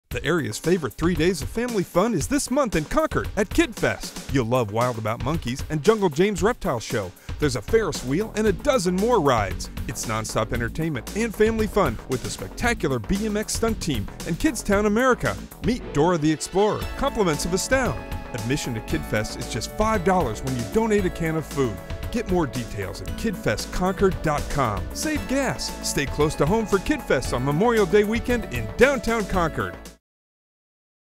Bay Area KidFest 2011
Memorial Day Weekend 2001 it's Kidfest in downtown Concord!  The place to be for fun, rides, shows and excitement.  Here's our new TV spot rolling out this week.